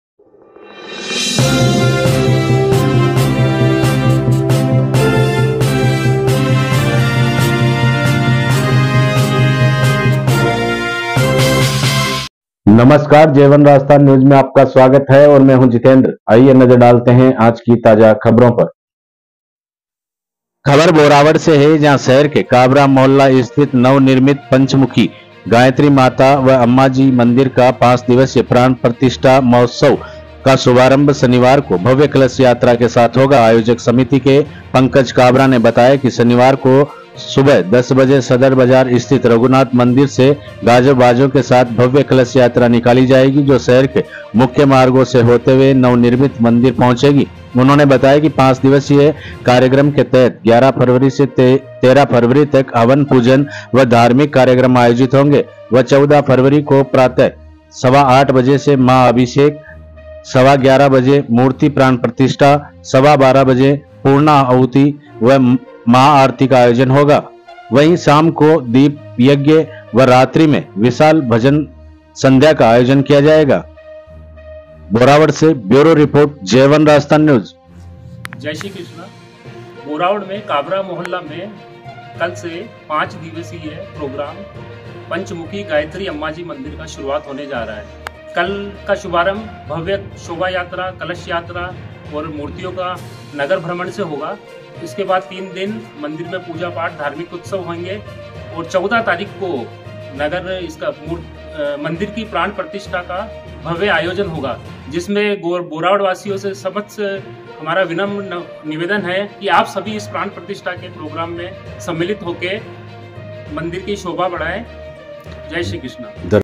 बोरावड़। श्री पंचमुखी गायत्री माता मंदिर प्राण प्रतिष्ठा महोत्सव का कल कलश यात्रा से होगा शुभारंभ।
@ ब्यूरो रिपोर्ट